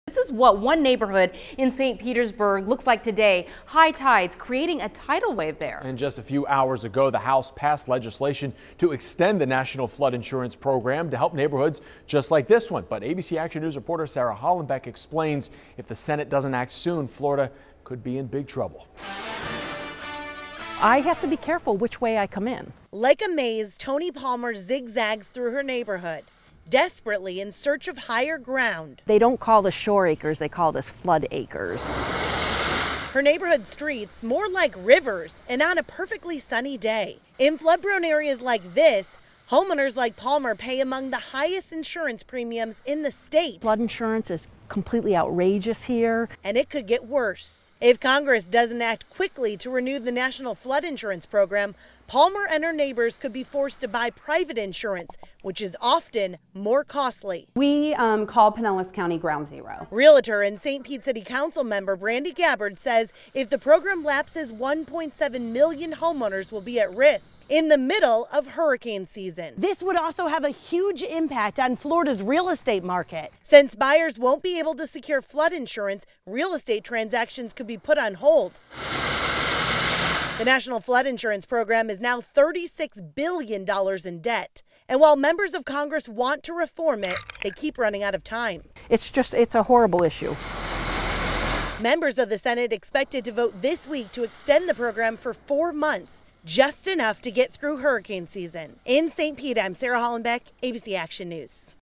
House passes flood insurance extension, now it's up to the senate
Now, people who live in flood-prone areas like St. Pete have another thing to worry about. The National Flood Insurance program expires in 6 days.